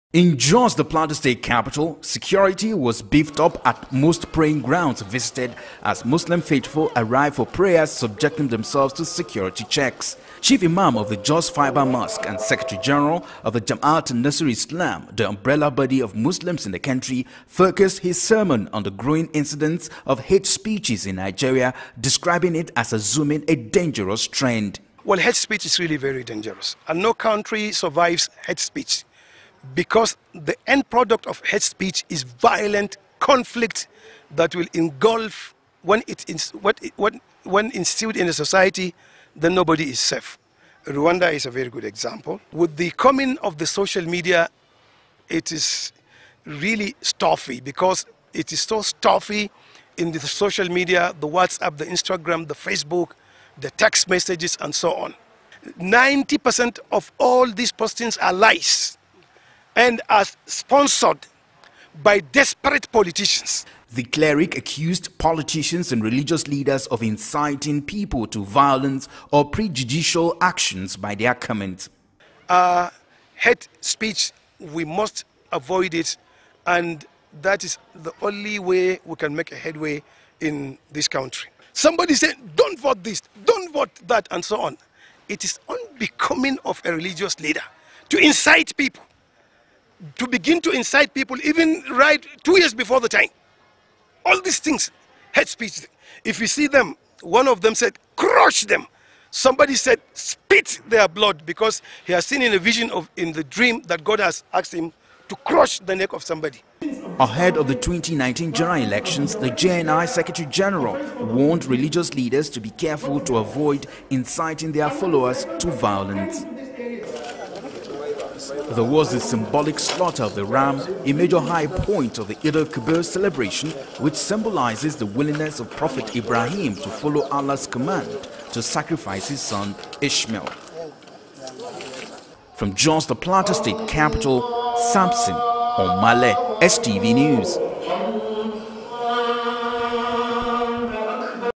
SALLAH IN JOS: Islamic Cleric Warns against Hate Speech
Secretary General of the Jama'atu Nasril Islam, The umbrella body of Muslims in the country Sheikh Khalid Abubakar Aliyu says the growing incidence of Hate speeches in Nigeria is assuming a dangerous trend.

Sheikh Khalid while speaking  Shortly after the Eid prayers at the Fibre Mosque in Jos on Tuesday accuesd politicians and religous leaders of inciteing people to violence or prejudicial action by their comments.